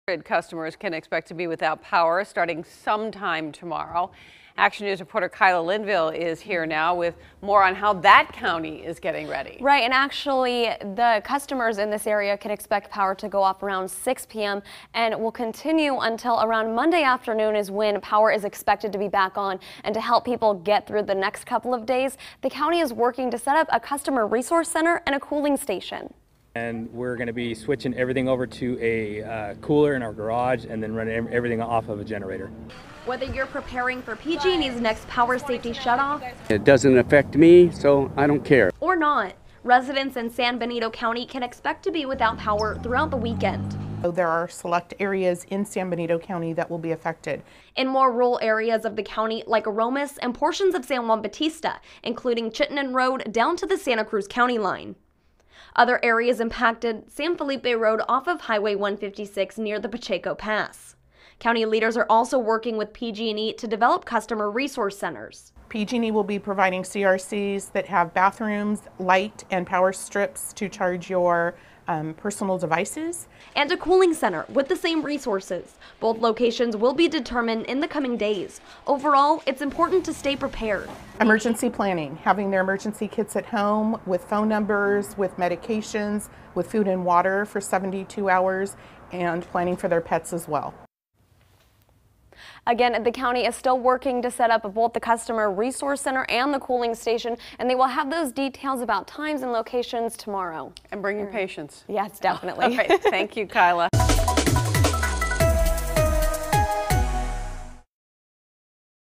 San Benito readies for heat, wind, PG and E power outages
With power outages expected to begin Saturday evening, SB county rushes to set up resource center and cooling stations